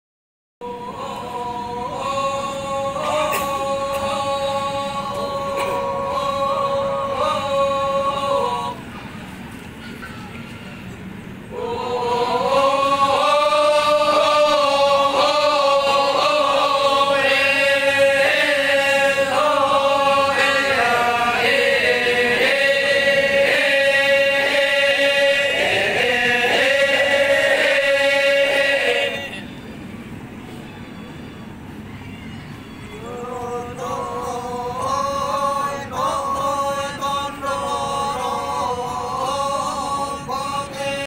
Ma'badong// Tarian/ Nyanyian Khusus Yang Harus Ada Dalam Upacara Rambu Solo'.
Ma'badong adalah sebuah tarian khusus dan nyanyian khusus yang punya arti lirik tersendiri dan di lakukan secara berkelompok serta membentuk lingkaran dan saling berpegangan tangan, biasanya mereka Makai baju seragam namun kadang juga bebas.
isi dari syair ma'badong, adalah menceritakan asal usul dari langit, masa kecilnya, amal dan kebaikannya serta semua hal yang menyangkut dirinya yang di anggap terpuji. selain itu syairnya juga mengandung harapan bahwa orang mati tersebut dapat memberkati keluarganya yang masih hidup dengan segala kebaikannya.